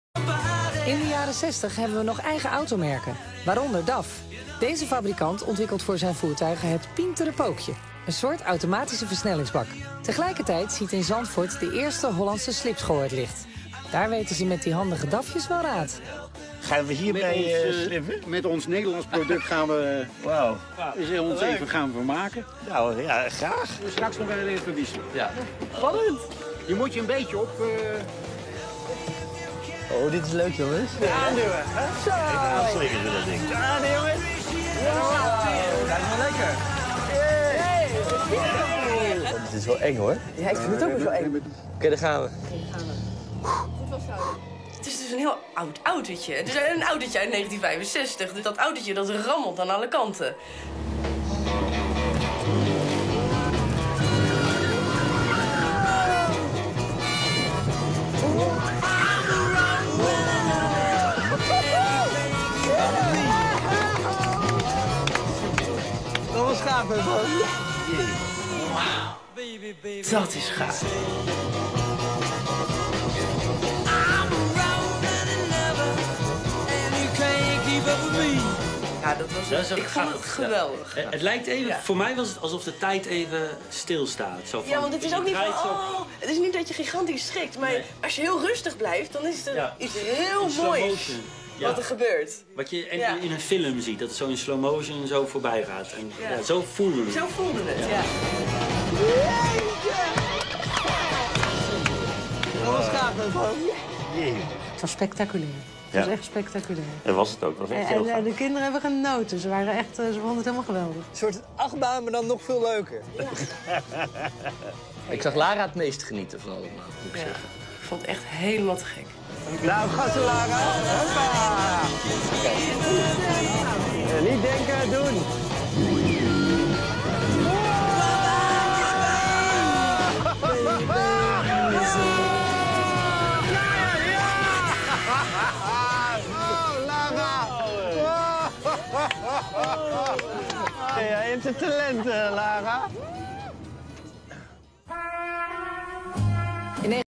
DAF dancing surprise
I found this on another website.  The commentary is in Dutch, but any DAF appreciator will understand.
Rob Slotemaker was one of DAF's racing drivers in the 1960s.  Here he takes the wheel of a 32 very recently, and shows some friends what can be done for fun.
Best not to try this yourself!